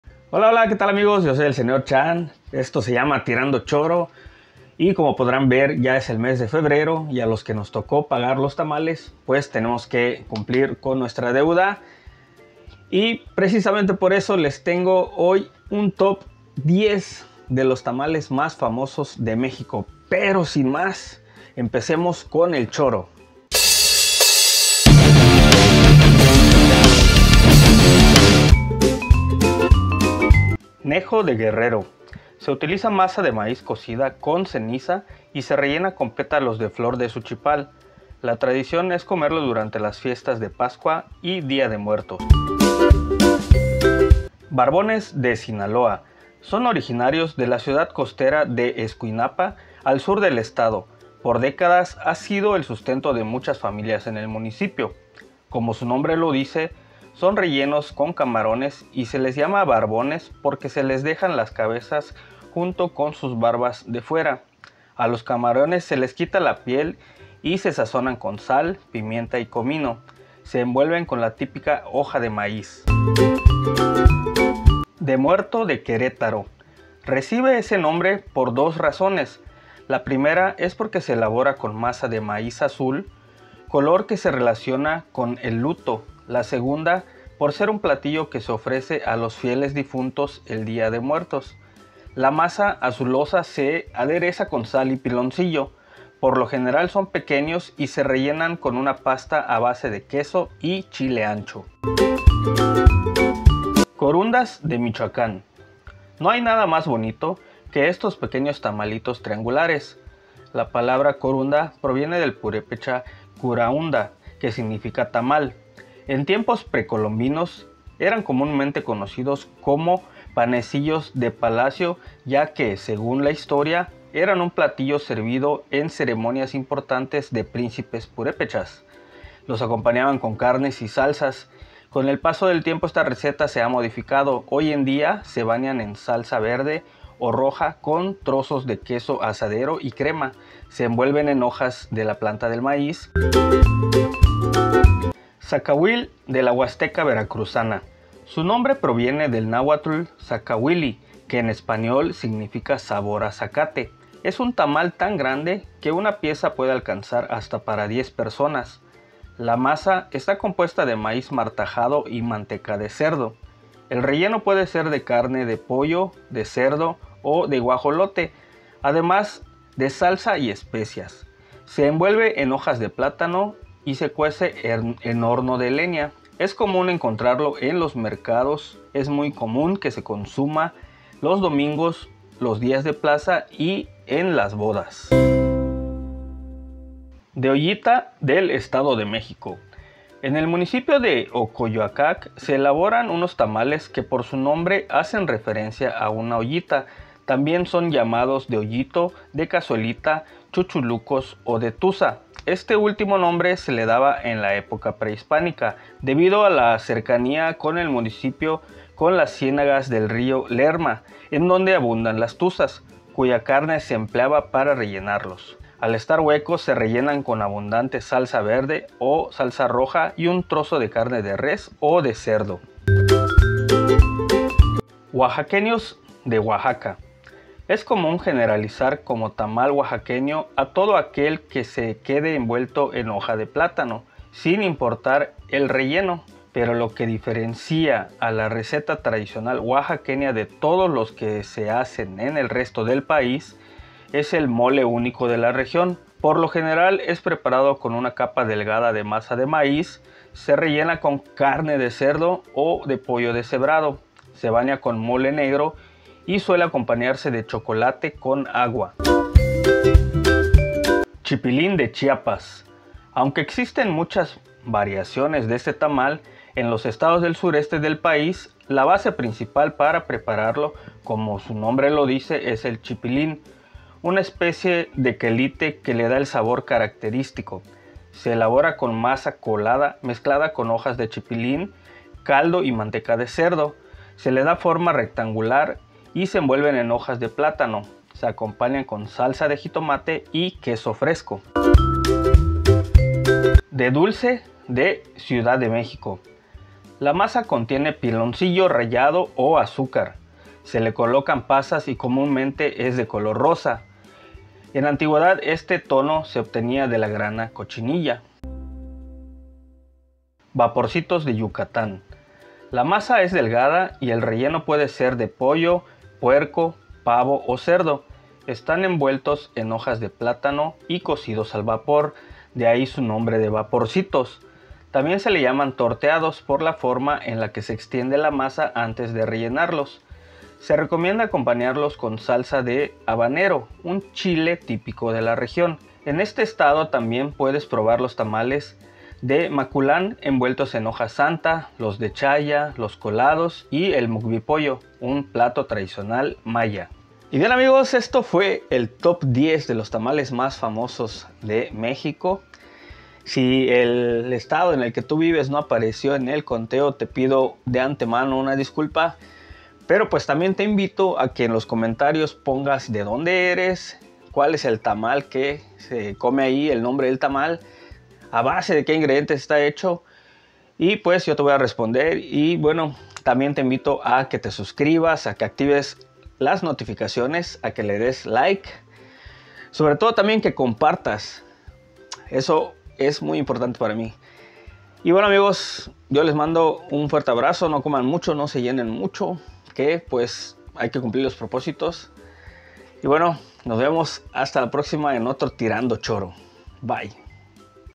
Top 10 de Tamales Mexicanos 😋- Tirando Choro
Hablar de tamales es hablar de toda una tradición ancestral que continúa vigente. Con el paso tiempo se ha fortalecido gracias a la diversidad cultural y festiva que hay en México.

Su origen data de la época prehispánica, su base principal es el maíz, no importa su tipo, aquí lo que varía es el relleno y las hojas con las que se envuelven: plátano, maíz y maguey, entre otras.

Nos vemos en otro video de 🎥"Tirando Choro"🎬
Bytes!!!